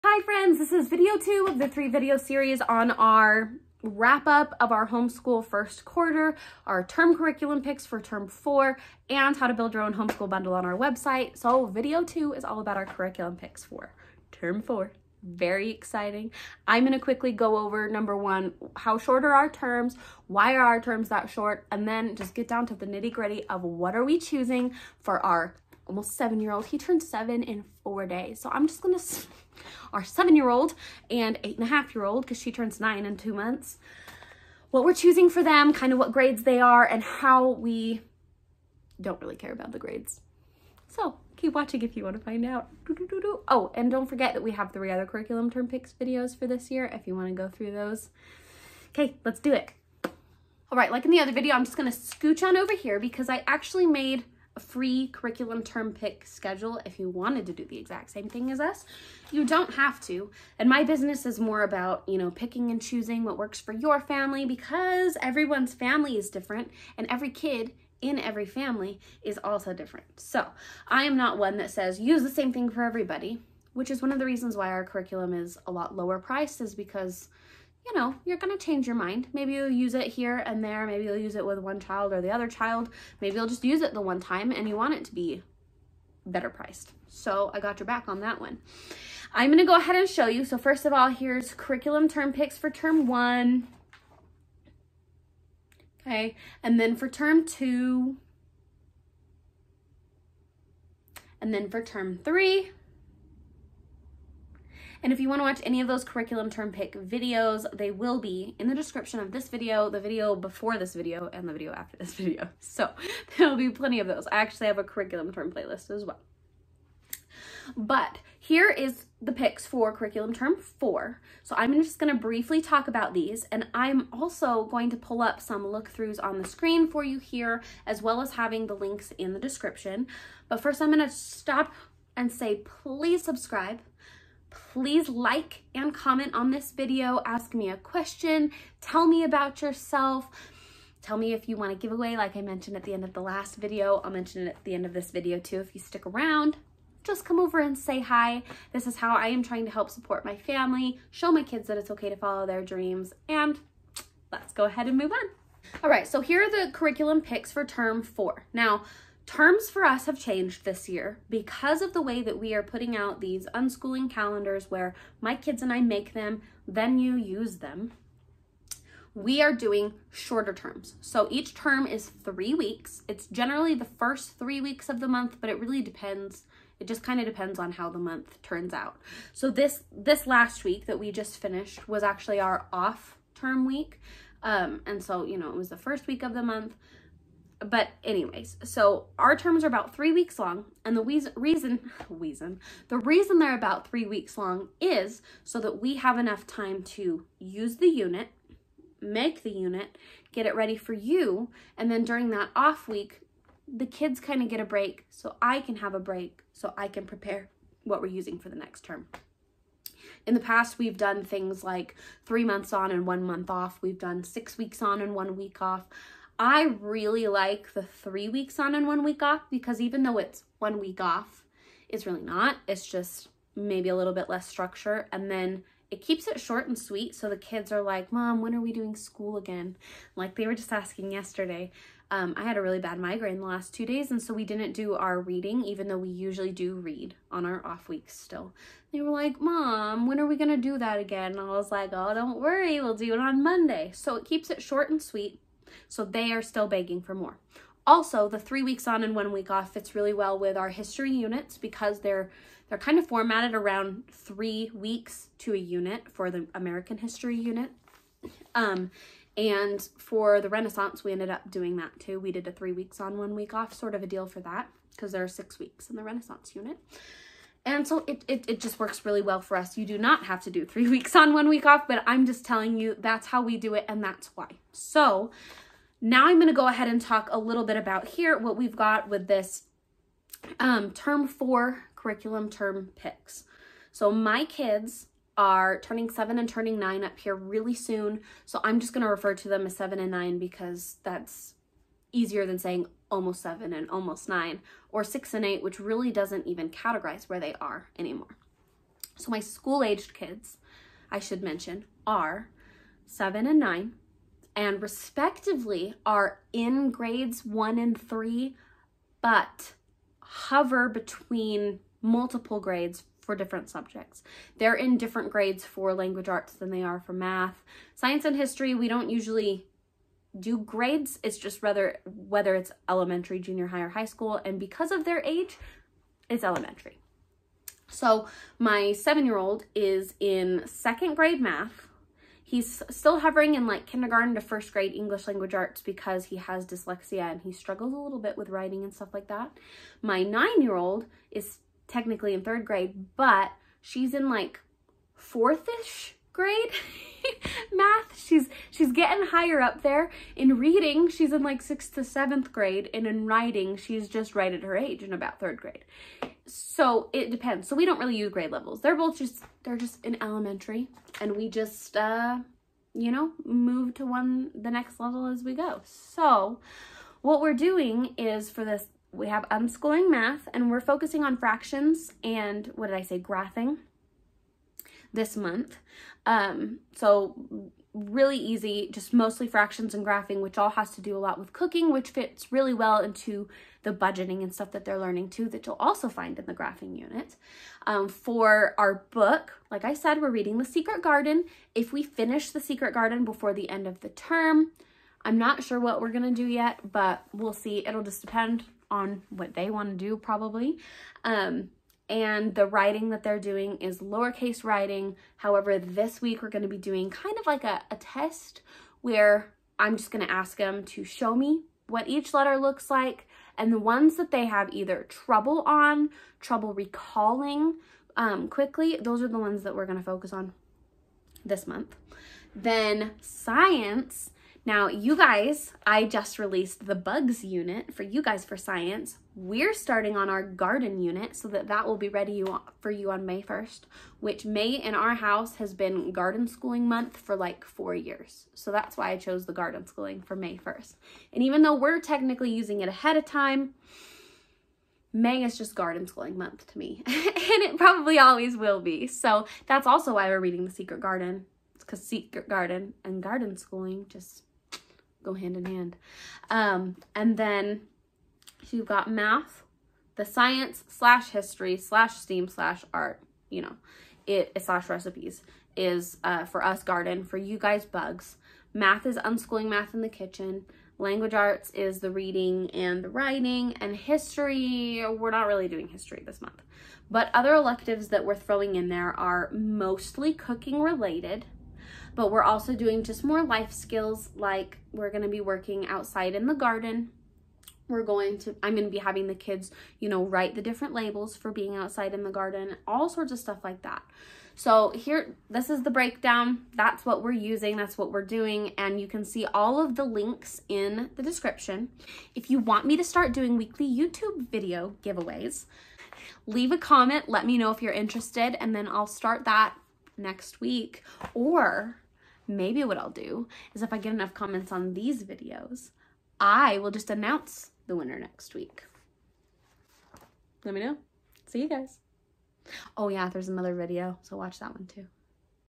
Our Curriculum Picks for Term 4 of 2023 | 7 and 9 Year old | Love at Home Education
2023 Term 4 Homeschool Curriculum Picks- 7 and 9 year old | Love at Home Education

Are you looking for the perfect homeschool curriculum for your elementary student in their next semester? Get the scoop on the top picks every parent should consider!

With so many homeschooling options available, finding the right curriculum for your elementary student can be overwhelming. To make your life easier, we’ve put together a comprehensive list of the top picks for homeschoolers this term Get excited to find the perfect homeschooling curriculum for your kid’s term 4!

Jump ahead:
0:20 All about our terms
1:31 free curriculum pick planner
2:00 curriculum term 4
2:36 shorter terms
4:41 actual picks & grades
6:06 picks
7:39 recap

✅Resources mentioned for term 4:
❤️Morning work: April Traceable Calendars
❤️Read Aloud and Writing: The Ultimate Unit Study: Gardenschooling Edition

***When you make a purchase through some of the links below we make a small percentage at no additional cost to you.